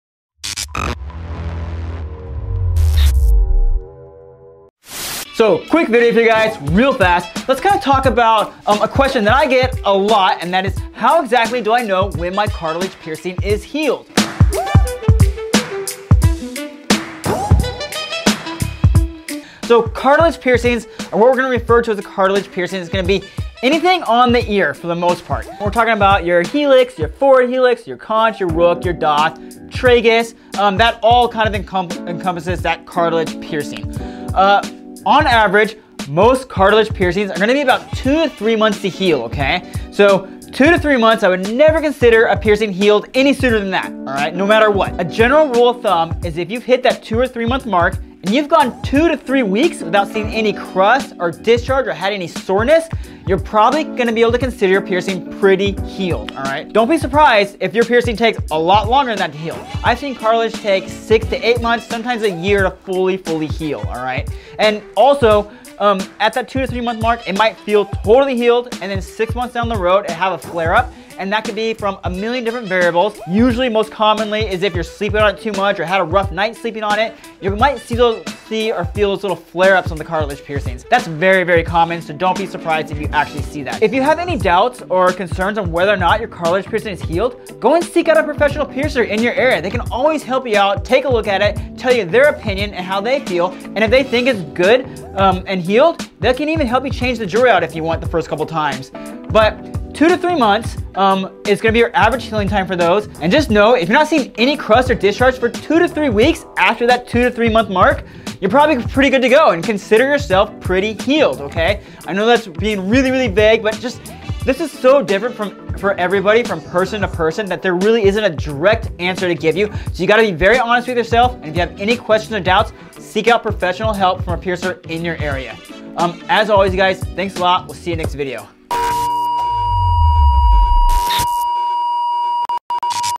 How To Tell When a Cartilage Piercing is Healed? | UrbanBodyJewelry.com
In another short segment video Matt Southwood goes over how to tell when a cartilage piercing might be healed. Matt is a professional piercer in San Luis Obispo, California with 20+ years of experience.